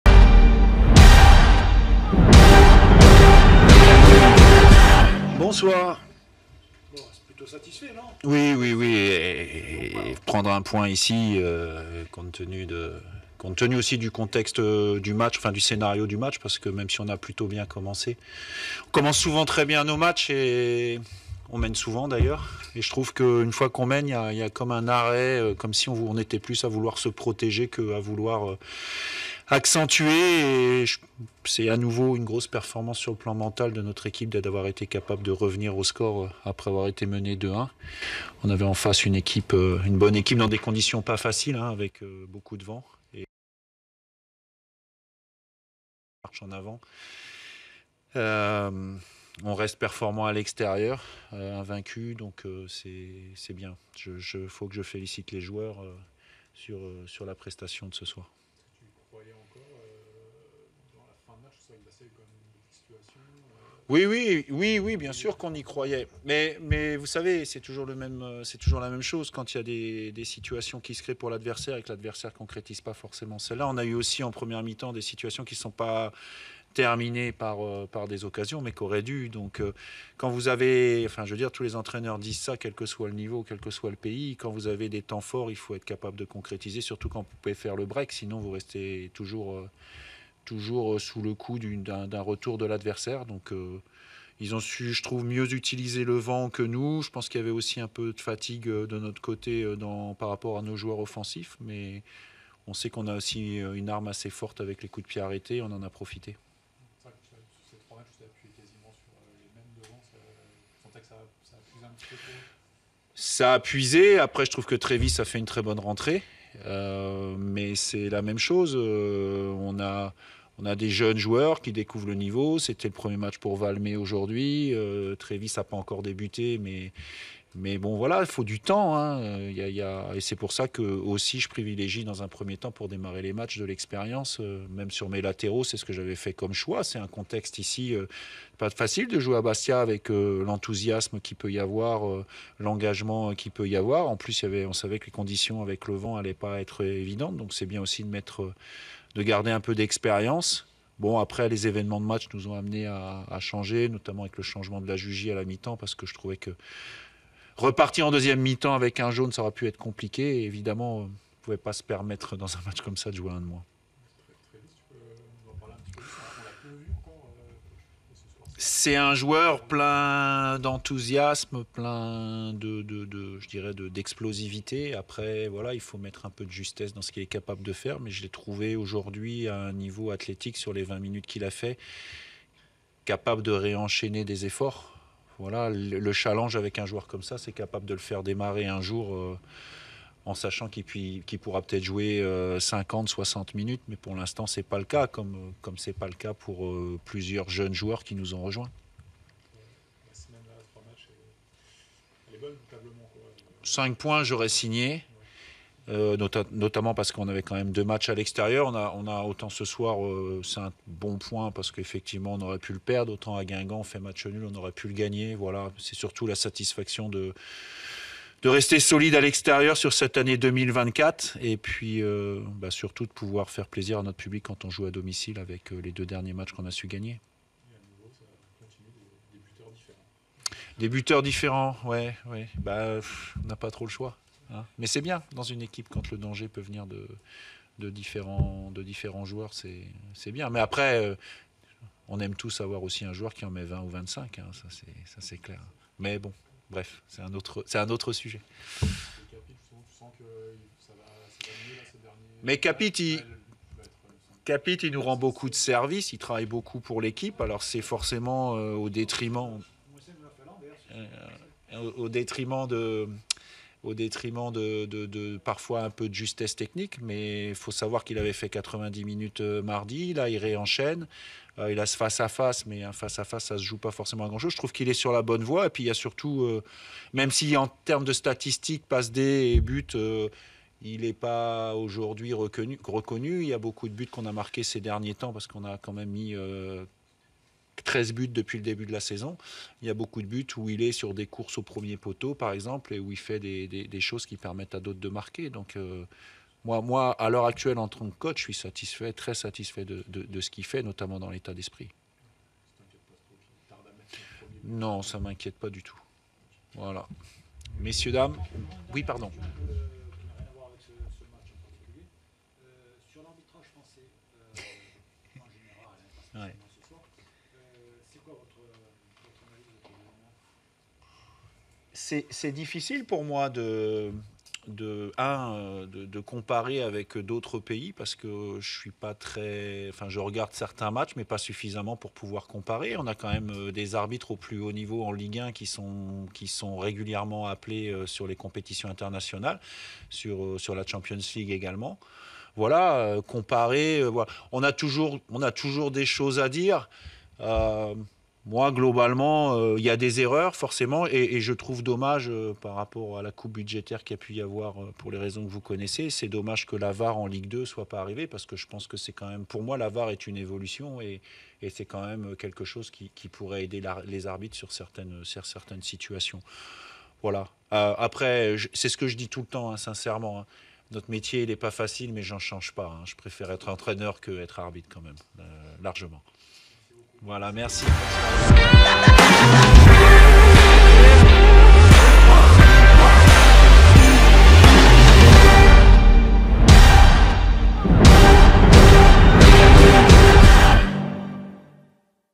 J7 - Laurent Guyot (conférence de presse après-match)
🔴 SC Bastia - FC Annecy : Laurent Guyot aux micros des journalistes après avoir obtenu le point du nul à Bastia.

👍 Laissez votre avis sur la vidéo en commentaires ou avec les likes/dislikes !